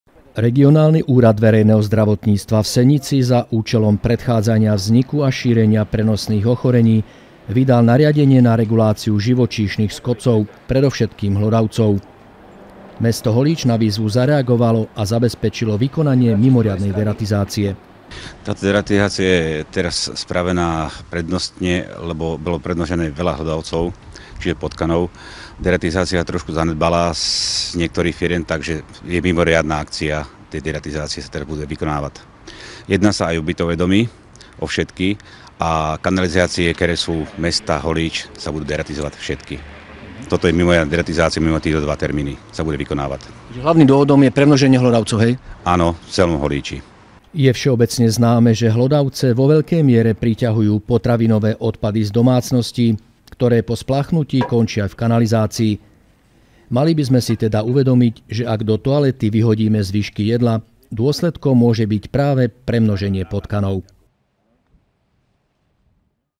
V meste sa vykonáva mimoriadna deratizácia
Mesto Holíč reagovalo na nariadenie Regionálneho úradu verejného zdravotníctva v Senici a vykonalo za účelom predchádzania vzniku a šírenia prenosných ochorení deratizáciu hlodavcov